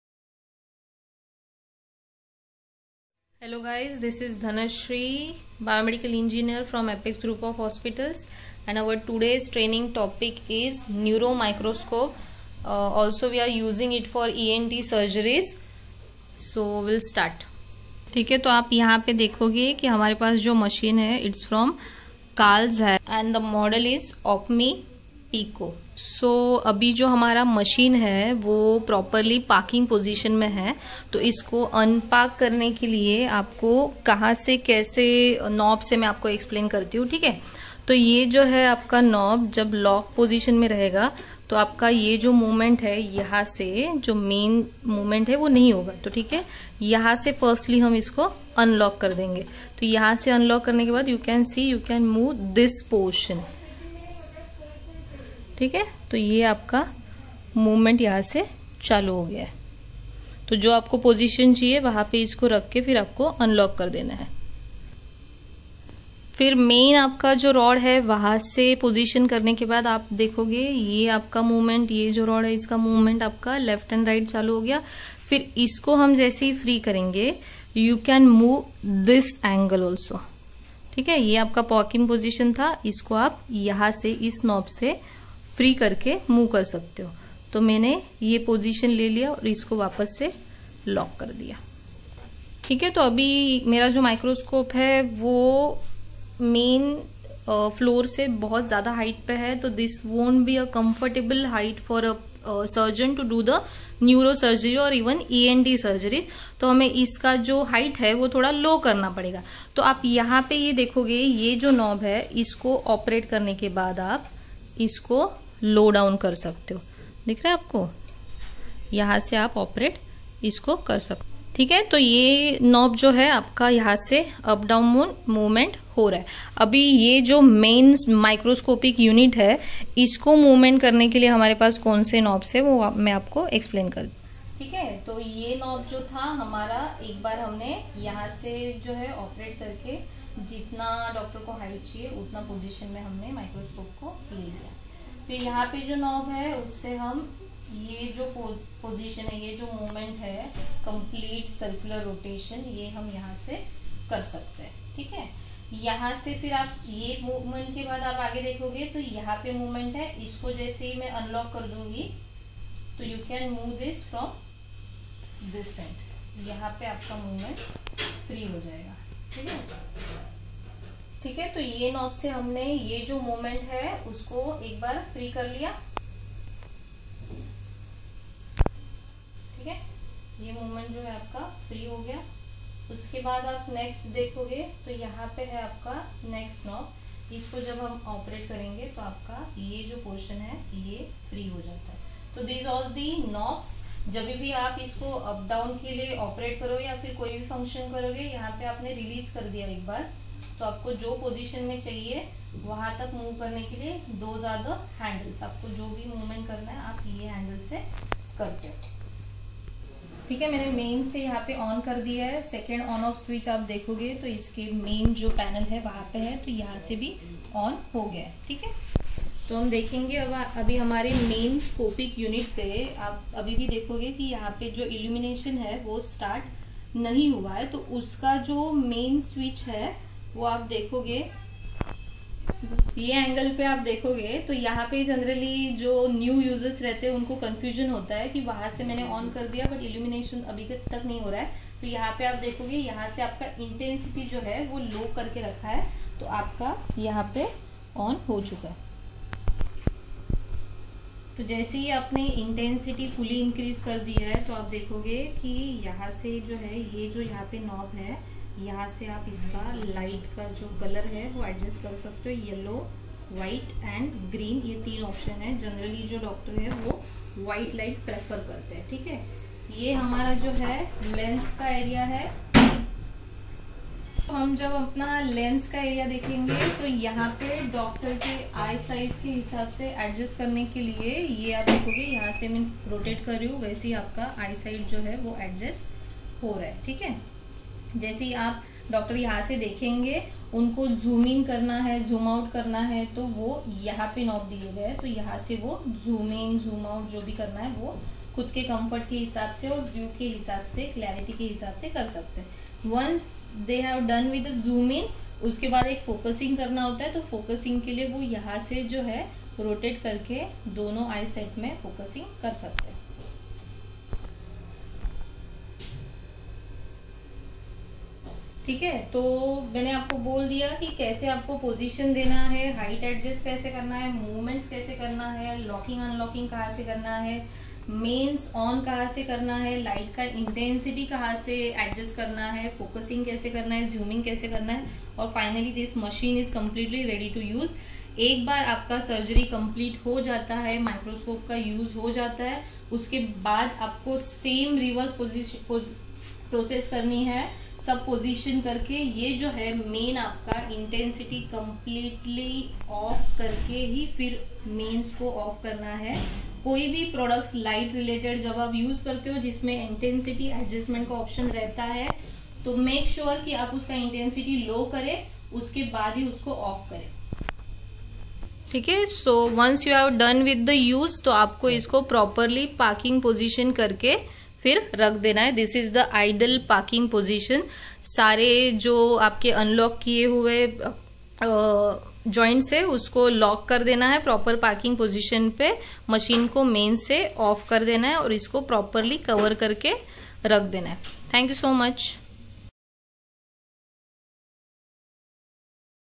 Training video for Carl zeiss neuro Microscope | Apex Hospitals
Training video for Carl zeiss neuro Microscope By Expert at Apex Group Of Hospitals